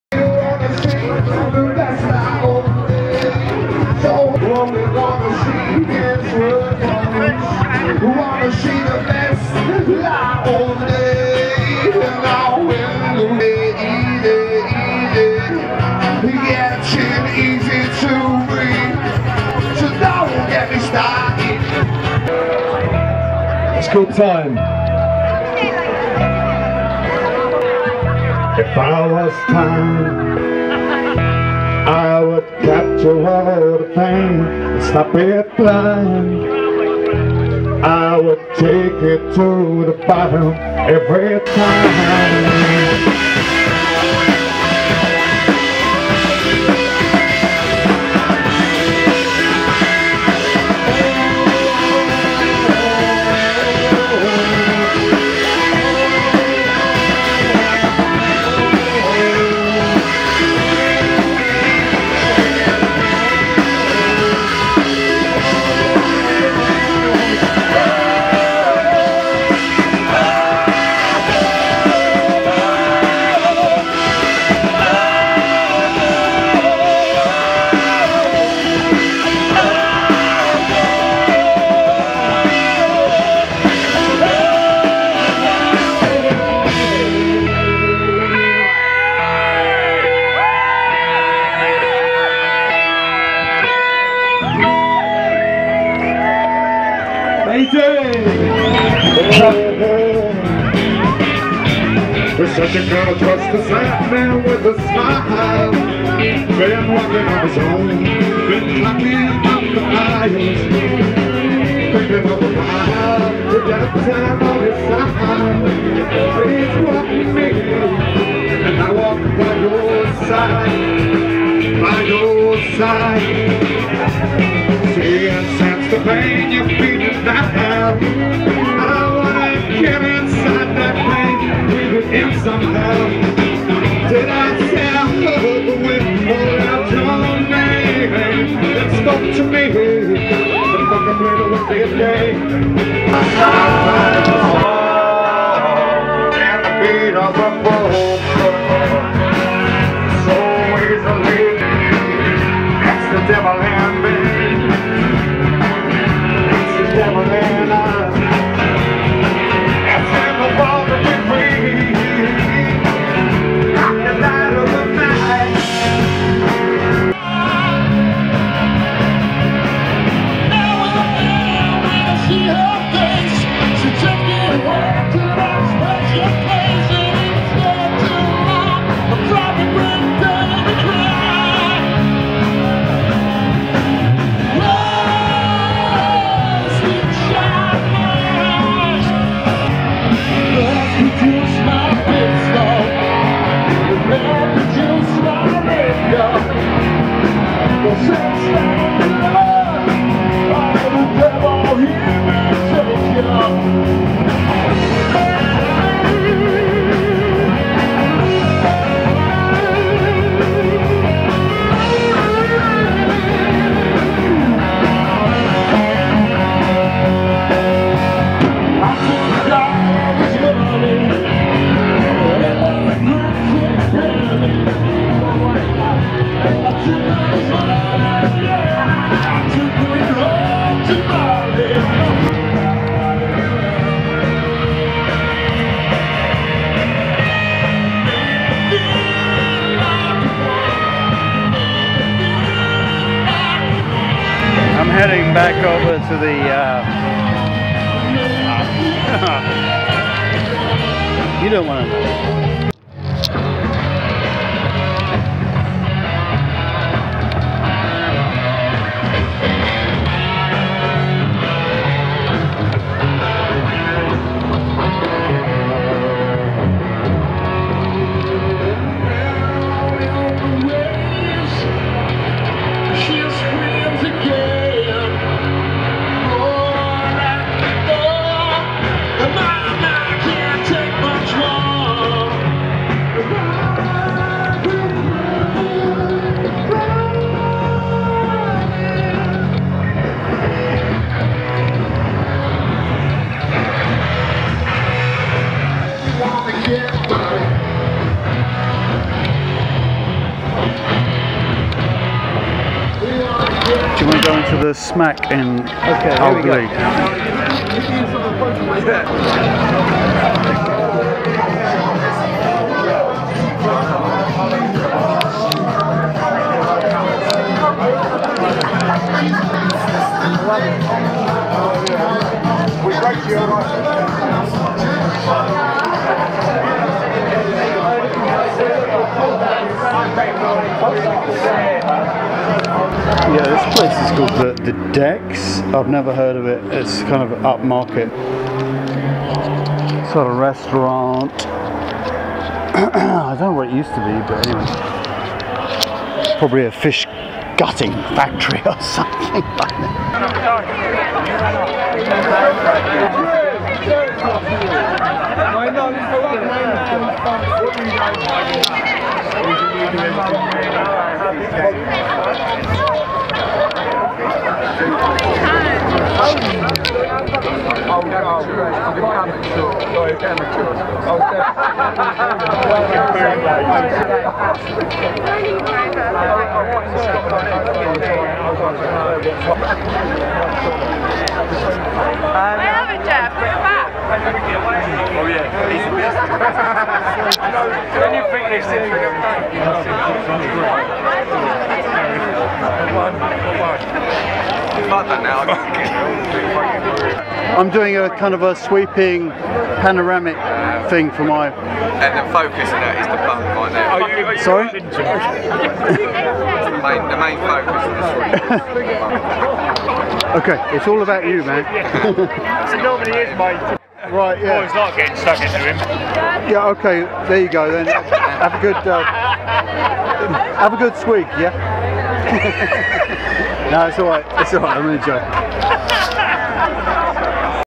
Leigh Folk 'Festival' 2014
Don't expect bells,accordians and frumpy neckerchiefed Pagans. Check the thumbnails,right,to see that.  Not really about the fest... just crawling my way around the pubs and checking out any live music.